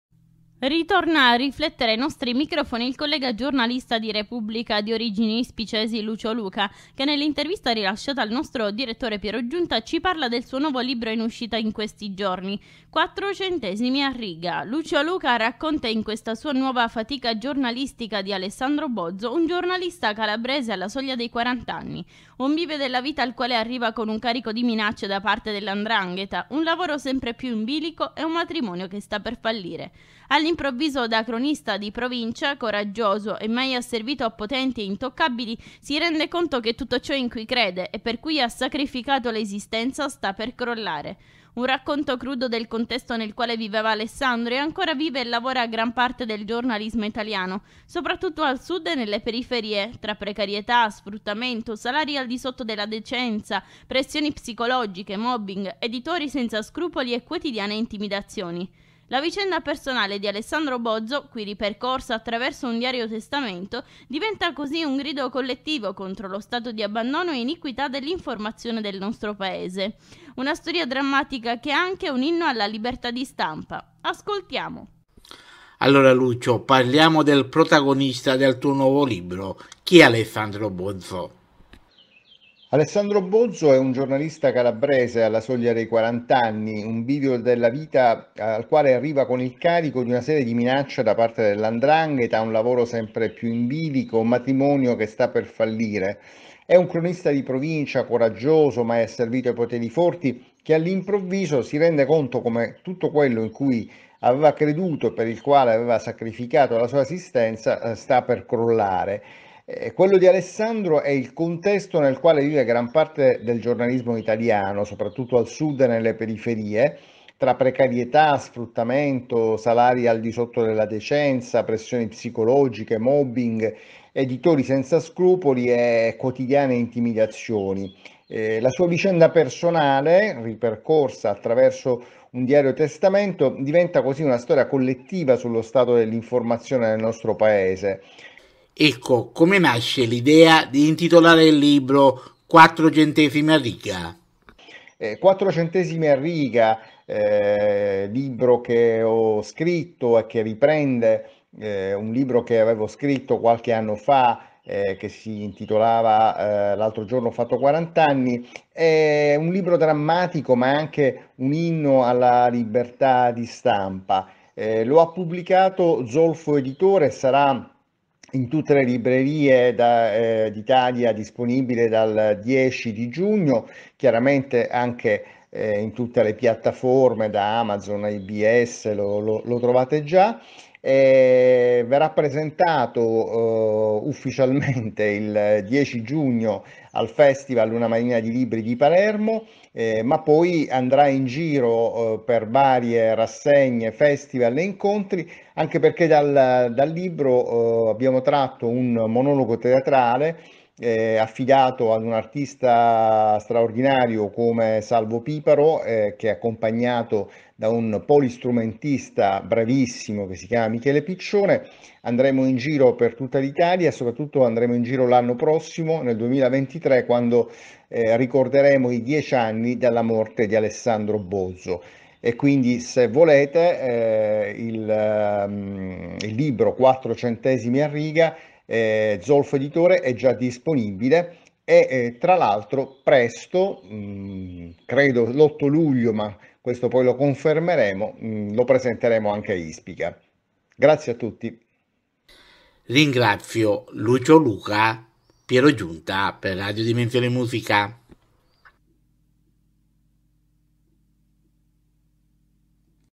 R.D.M. - QUATTRO CHIACCHIERE RADIOFONICHE CON IL GIORNALISTA LUCIO LUCA - "QUATTRO CENTESIMI A RIGA"
R.D.M. - L'intervista al collega giornalista di origini ispicesi di "Repubblica", Lucio Luca in occasione del suo nuovo libro "Quattro centesimi a riga".

Ritorna a riflettere ai nostri microfoni il collega giornalista di “Repubblica” di origini ispicesi Lucio Luca che nell’intervista rilasciata al nostro Direttore Piero Giunta ci parla del suo nuovo libro in uscita in questi giorni  “Quattro centesimi a riga”. Lucio Luca racconta in questa sua nuova fatica giornalistica di Alessandro Bozzo, un giornalista calabrese alla soglia dei quarant’anni. Un bivio della vita al quale arriva con un carico di minacce da parte della ’ndrangheta, un lavoro sempre più in bilico e un matrimonio che sta per fallire. All’improvviso, da cronista di provincia coraggioso e mai asservito a potenti e intoccabili, si rende conto che tutto ciò in cui crede e per cui ha sacrificato l’esistenza sta per crollare.
Un racconto crudo del contesto nel quale viveva Alessandro e ancora vive e lavora gran parte del giornalismo italiano, soprattutto al Sud e nelle periferie: tra precarietà, sfruttamento, salari al di sotto della decenza, pressioni psicologiche, mobbing, editori senza scrupoli e quotidiane intimidazioni. La vicenda personale di Alessandro Bozzo, qui ripercorsa attraverso un diario-testamento, diventa così un grido collettivo contro lo stato di abbandono e iniquità dell’informazione nel nostro Paese.
Una storia drammatica che è anche un inno alla libertà di stampa. Ascoltiamo.